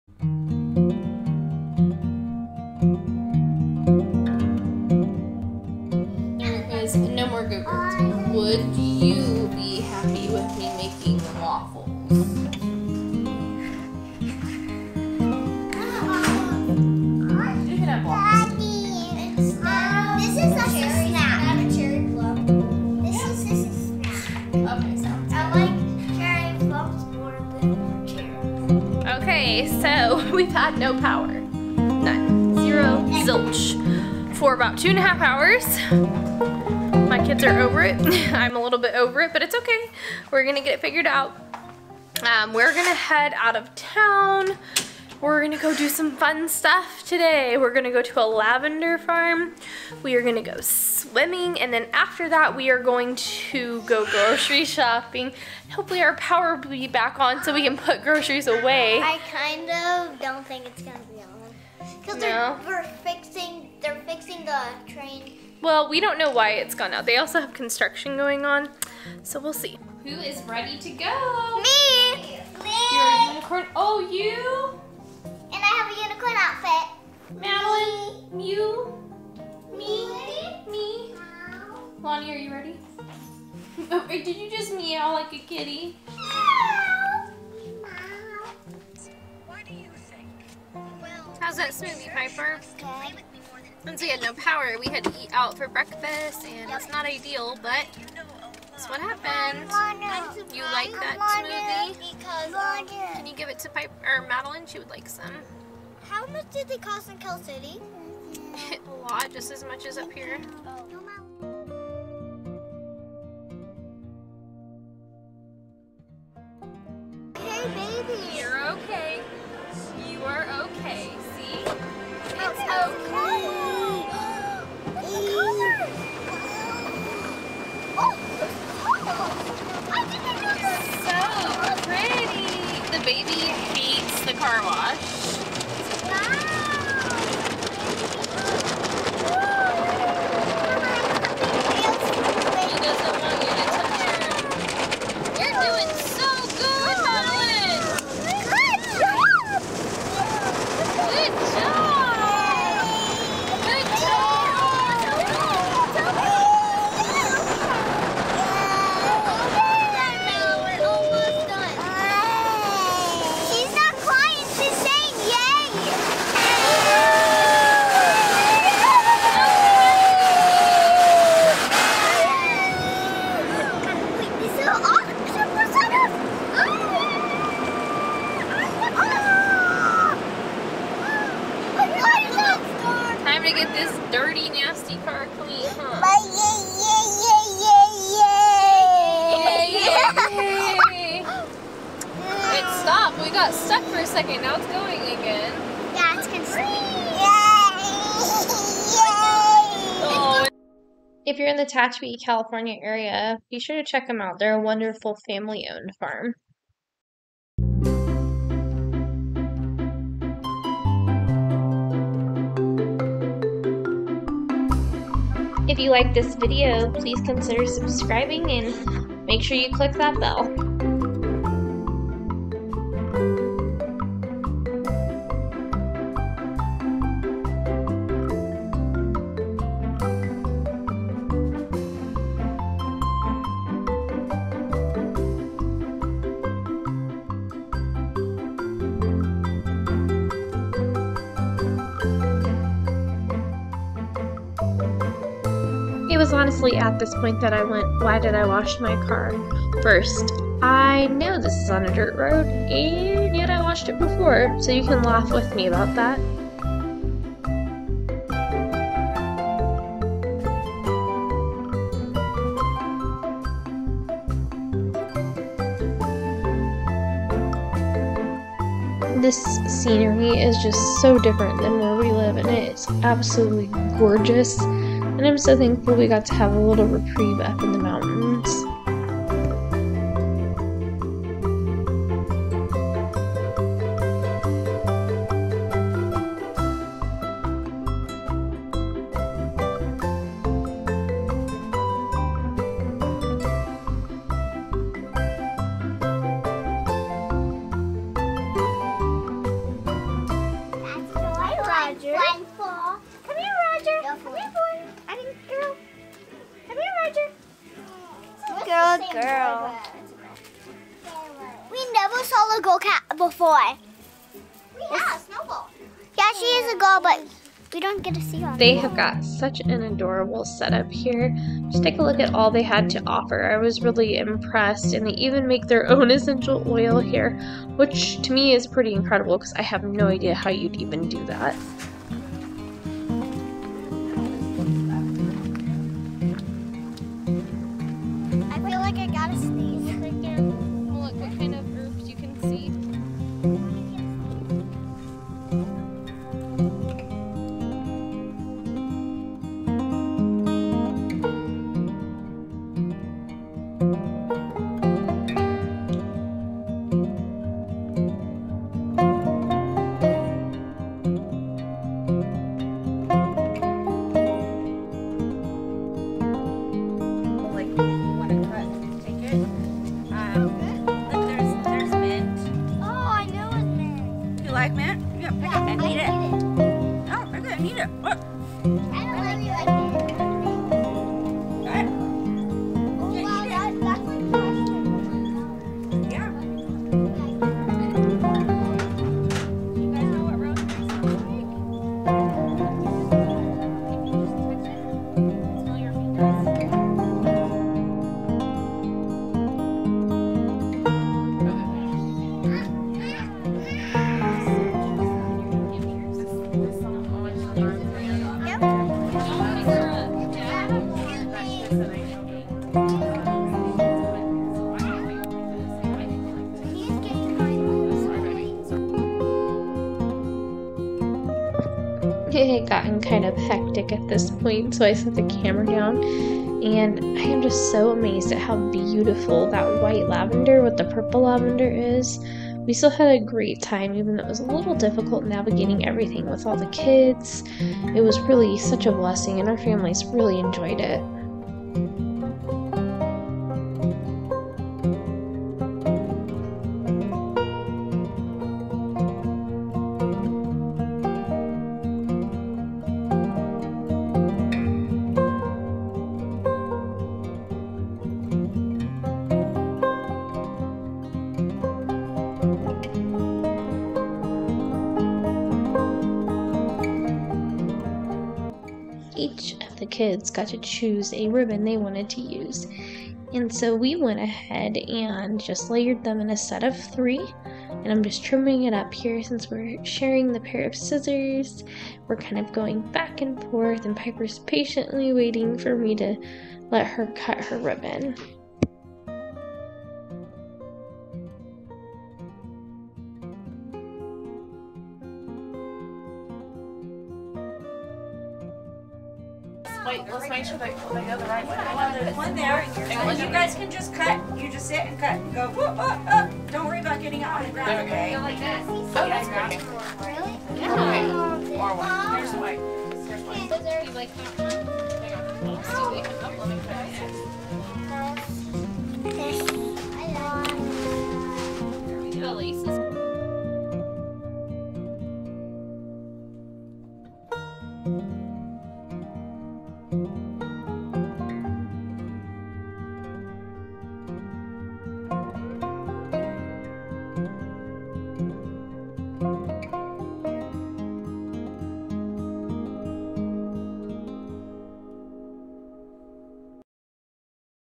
How to use Lavender. It's good for 20 years.
Lost power, car washes, dirt roads and beautiful bundles of lavender filled our senses today.

GPS drop for this fun little lavender farm.

Also on Facebook you can find us here.

Lastly our other channel is homeschool that works. Be sure to check it out.